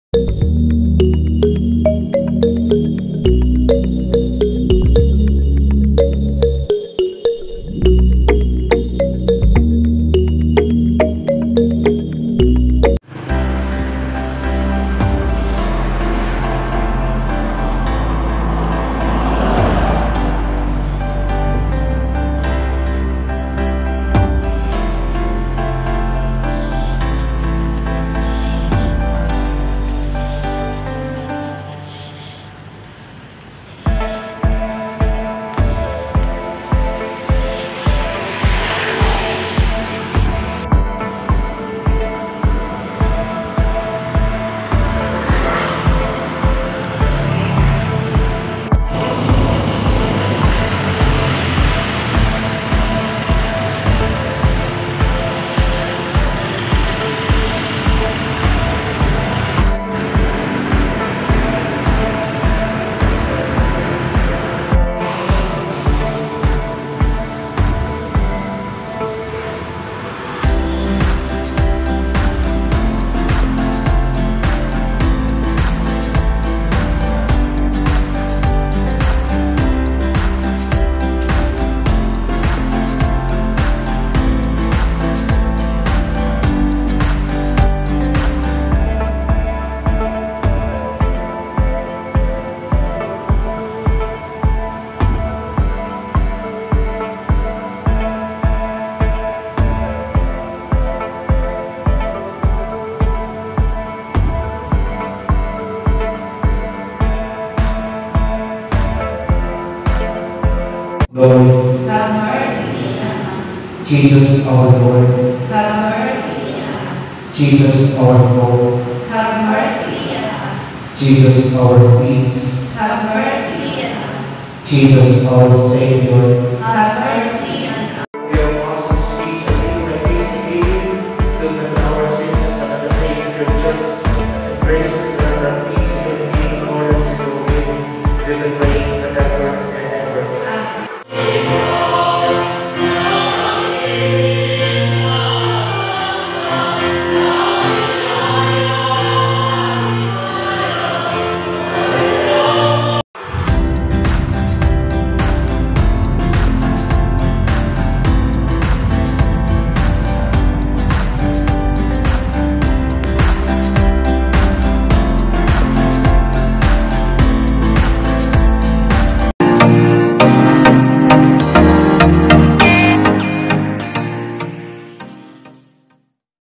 Walk and Pray
masaya maglakad para mag exercise at makita ang kagandahang nilikha't kaloob ng Diyos.